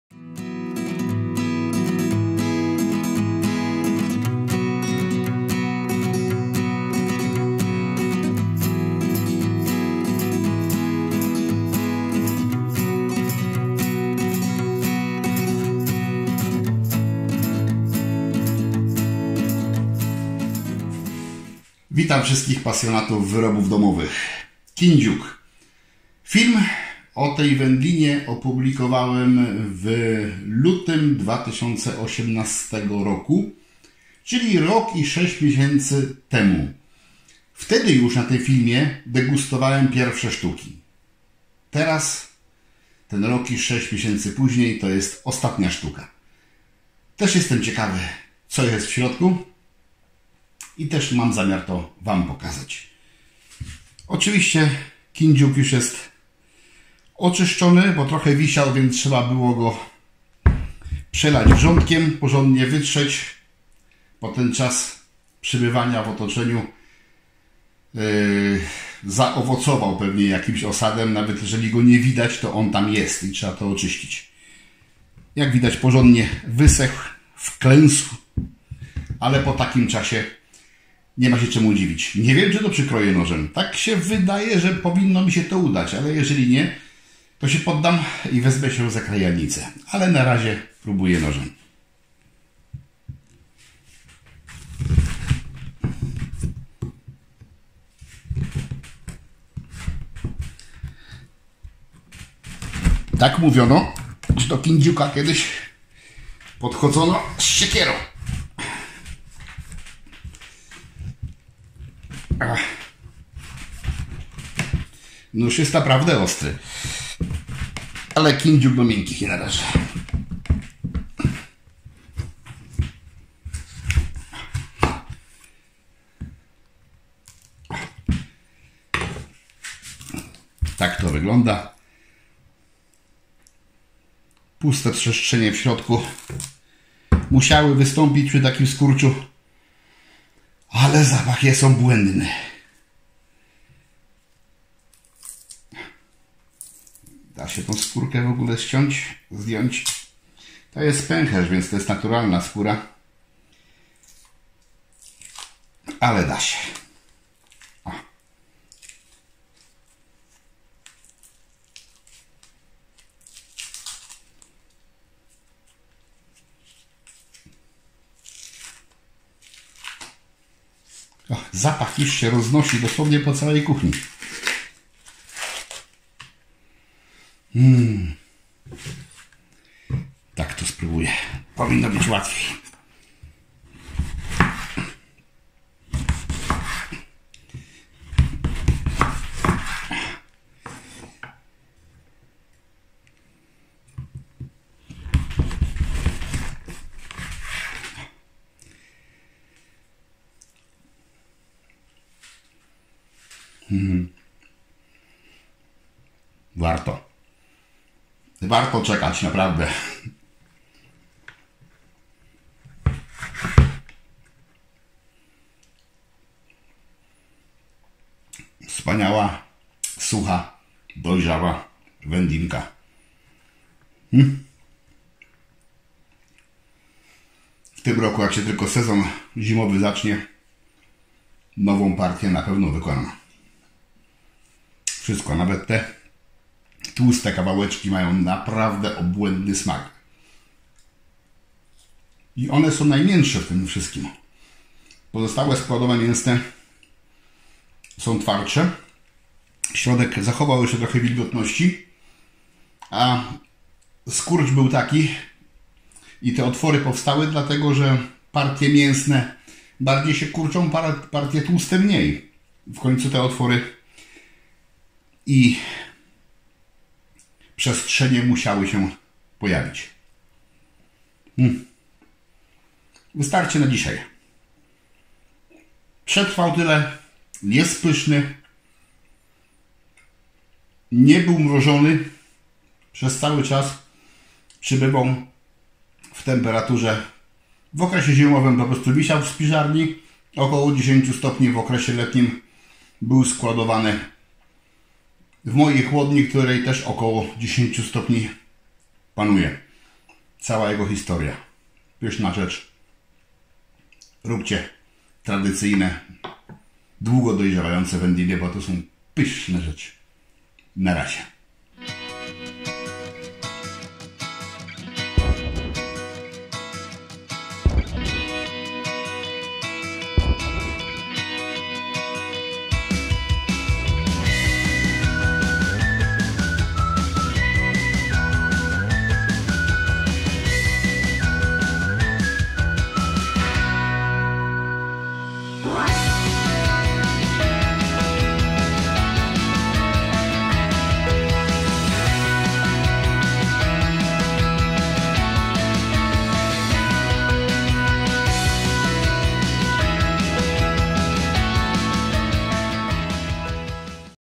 Kindziuk 1 rok i 6 miesięcy - Praktyka u Praktyka
Wędlinę tę wykonałem ponad 18 miesięcy temu. Kindziuk wysechł mocno, ale przetrwał mimo, ze nie zawiera całej listy składowych typu E. Nigdy też nie był w lodówce. Przechowywałem go w chłodzie bliskim 10 st C i w średniej wilgotności.  50-60 %.
Pachnie i smakuje zacnie  :-)
Warto sobie przypomnieć ten przepis zwłaszcza że zbliża się jesień i będzie można rozpocząć produkcję tej szlachetnej wędliny jaką jest kindziuk.

Kupując w sklepie internetowym wspierasz kanał!